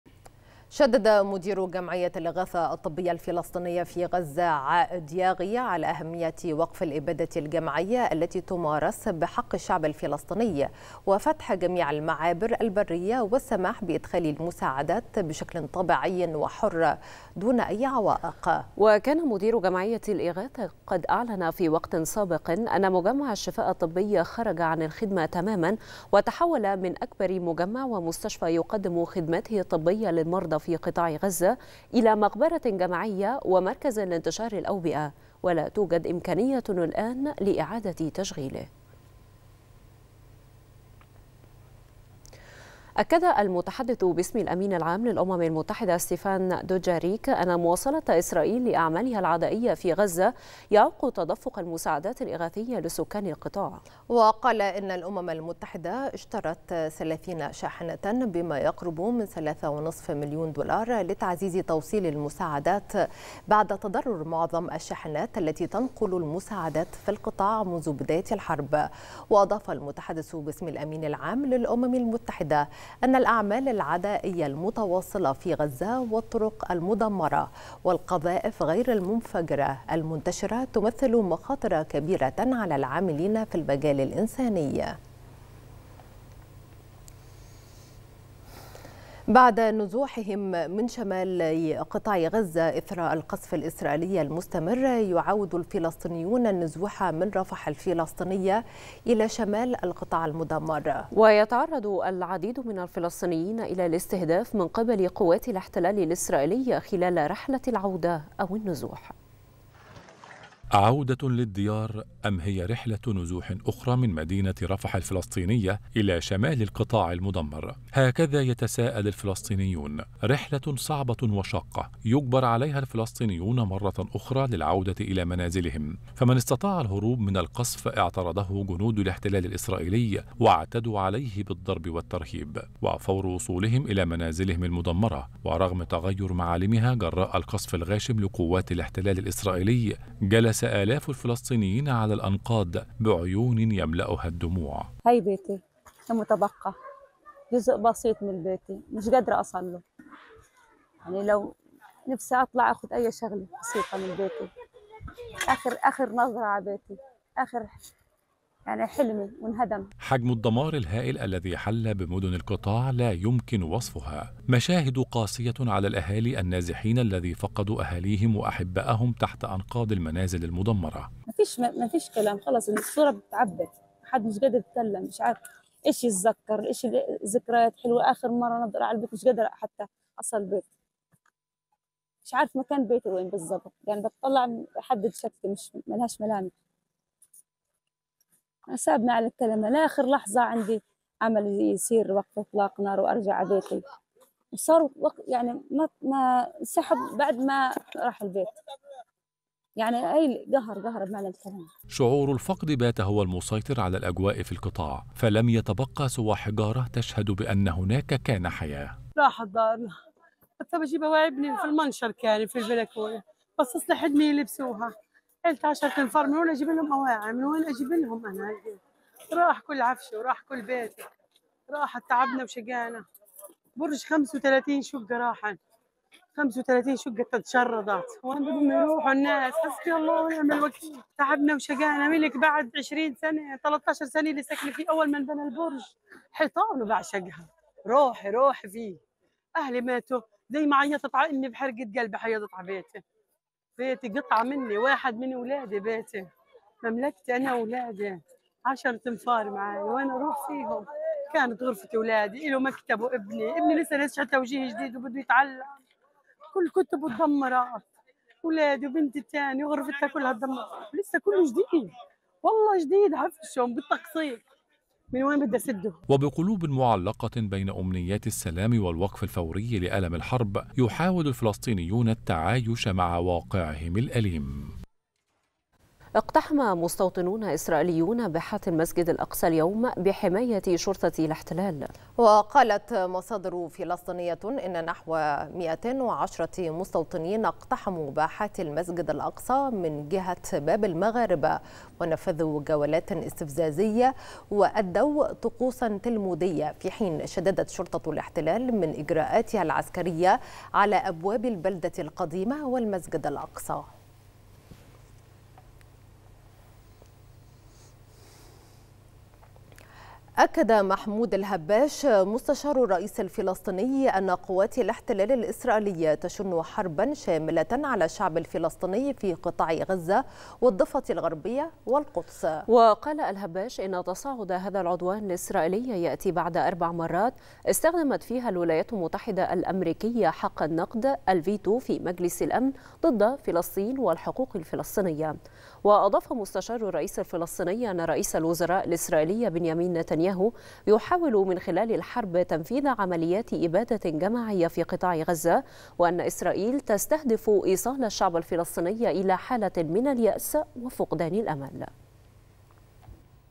"القضية الفلسطينية".. محمود الهباش: إسرائيل تشن حربا شاملة على غزة والضفة الغربية والقدس
نشرة الثالثة | "القضية الفلسطينية".. محمود الهباش: إسرائيل تشن حربا شاملة على غزة والضفة الغربية والقدس